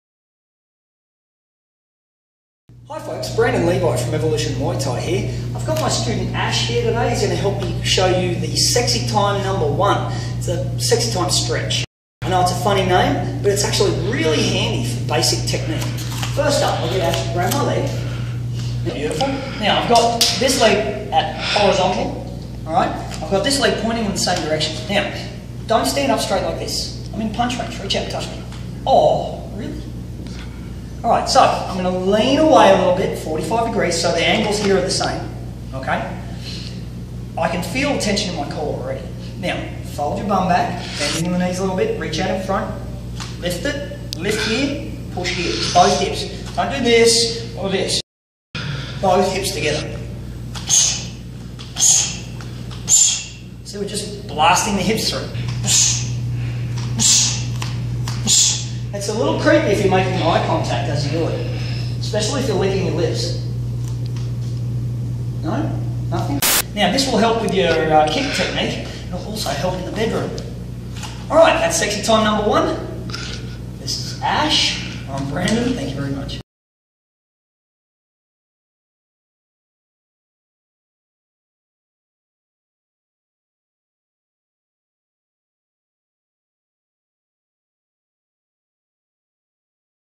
Muay Thai Kicking Drills - Sexy Time 1
No, it's not what you think it is (but it can be!).  Before you start kicking, here's a nice drill to get your body warmed up.

Presented by Kru Brandon Levi of Evolution Muay Thai